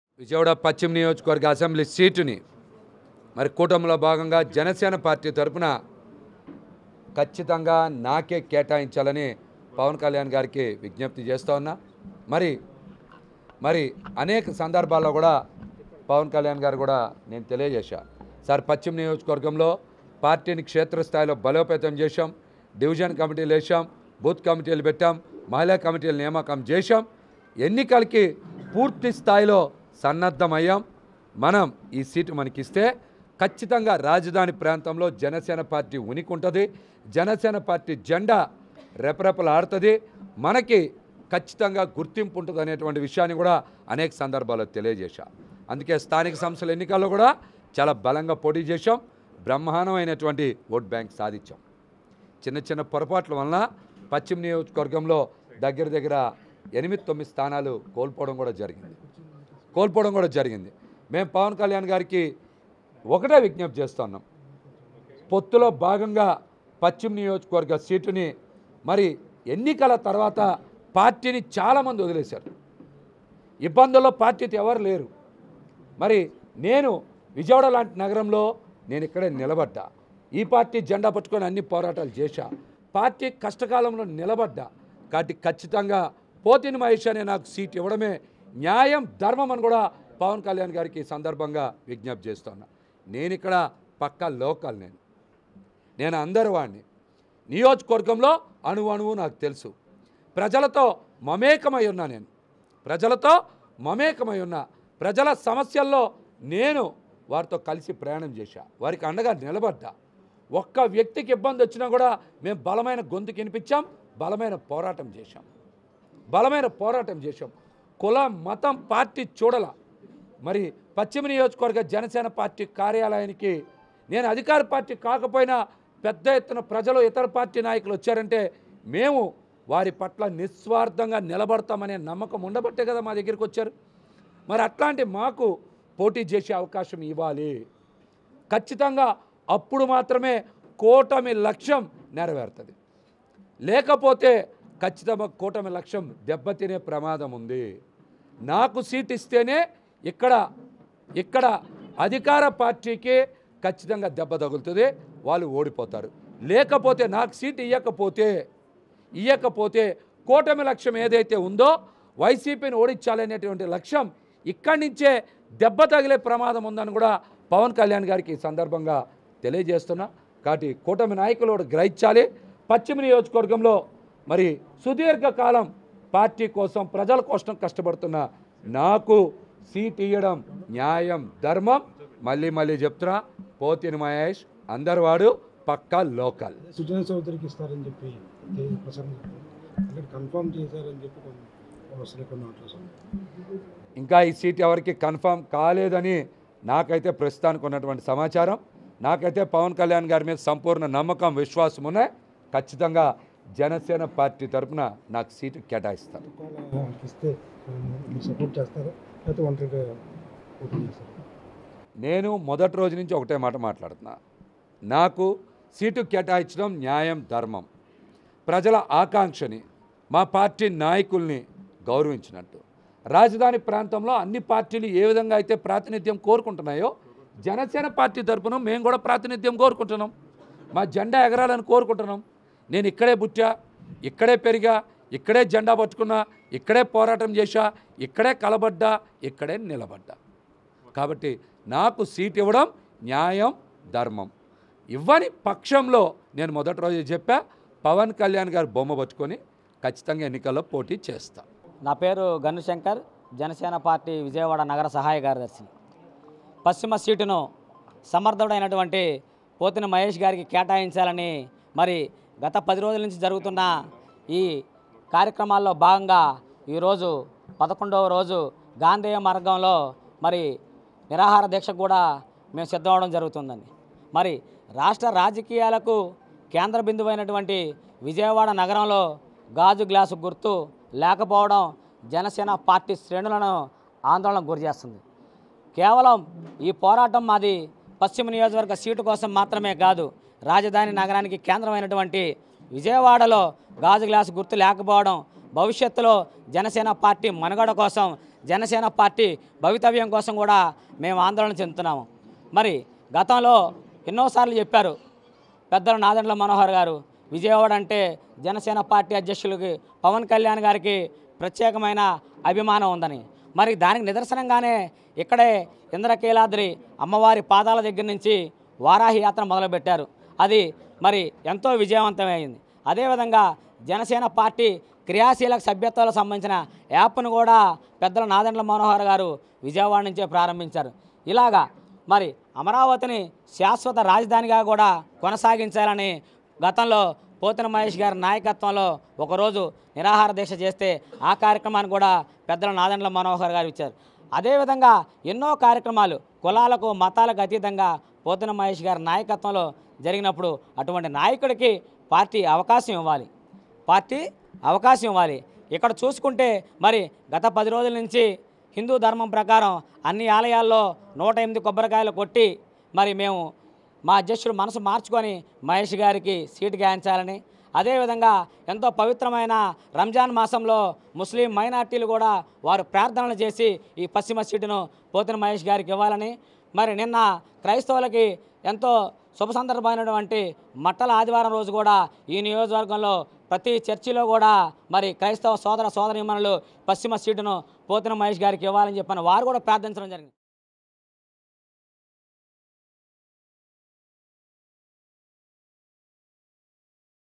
JanaSena Leader Pothina Mahesh Demands MLA Seat | Vijayawada | Pawan Kalyan || Samayam Telugu
Janasena party leader Pothina Venkata Mahesh demands Vijayawada west MLA seat for him.